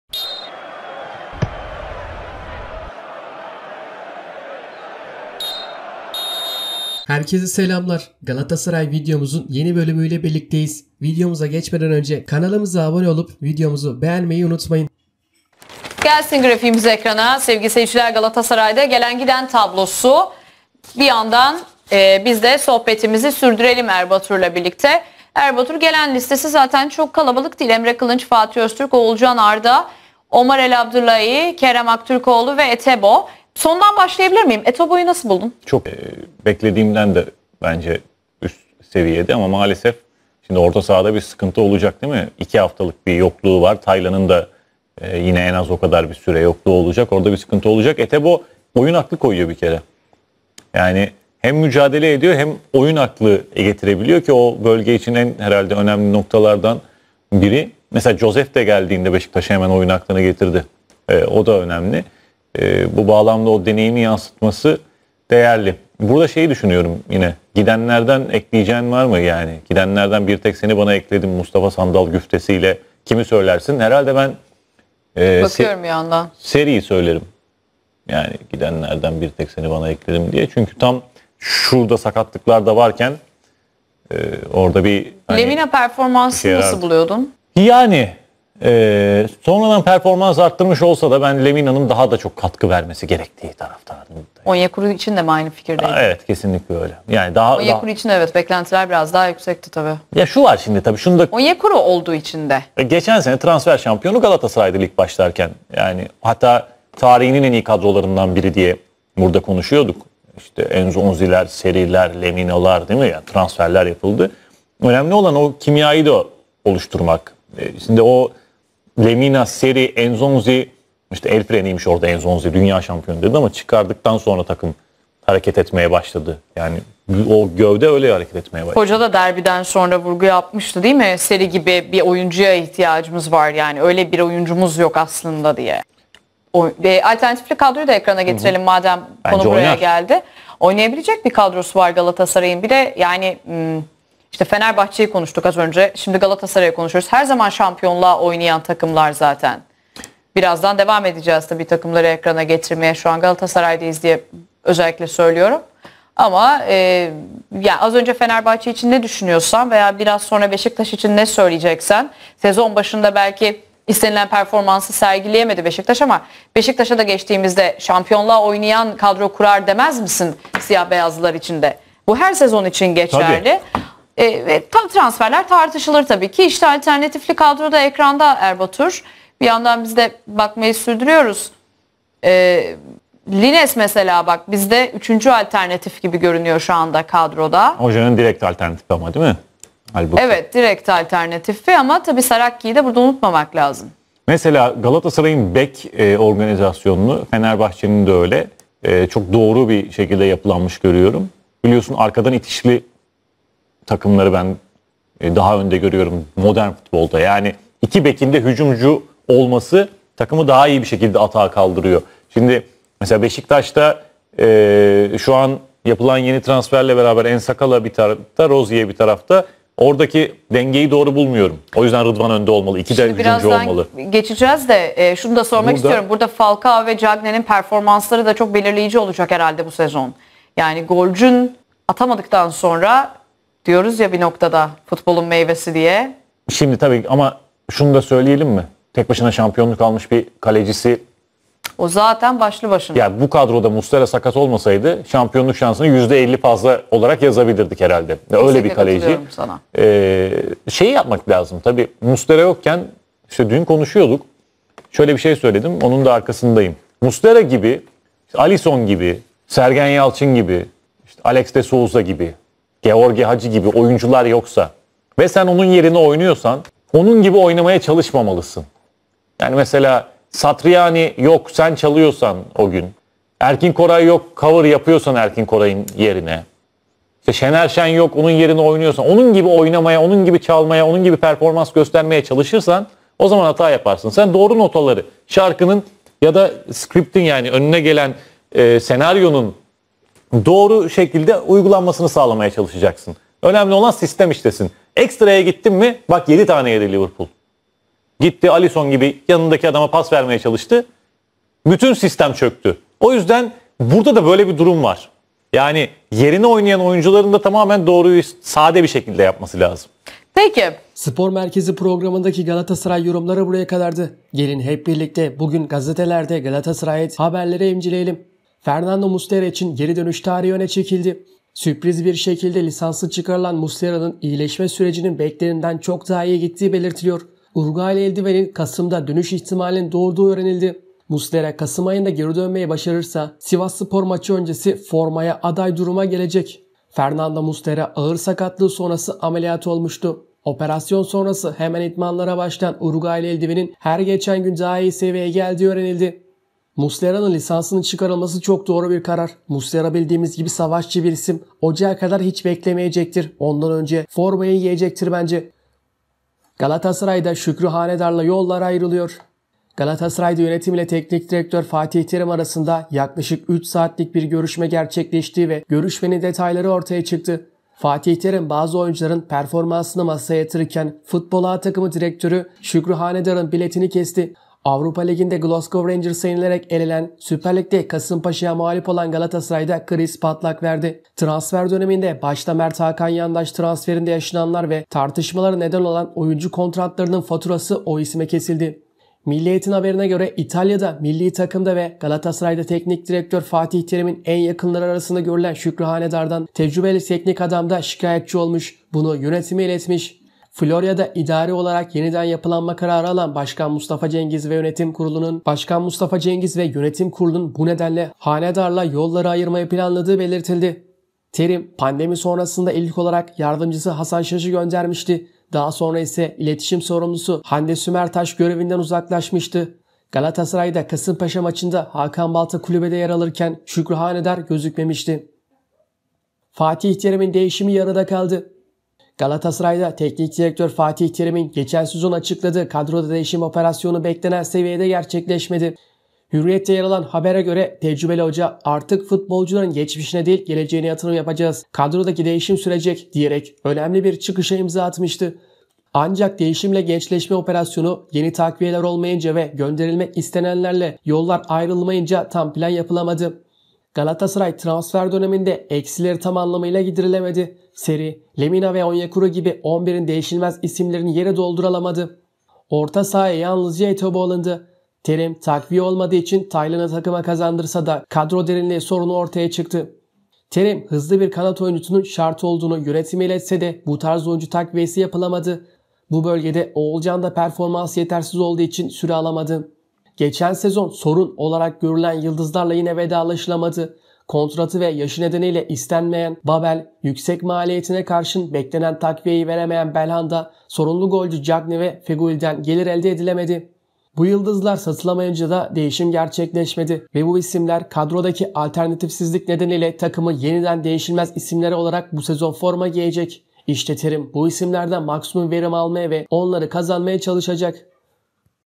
"Galatasaray Bu Kadroyla da Şampiyon Olur" l Erbatur Ergenekon'dan Galatasaray Yorumları
Erbatur Ergenekon ve Ceyda Büyükuzun Spor Merkezi programında Galatasaray hakkında yorumlarda bulundu.

İşte tüm gelişmeler ve dataylar..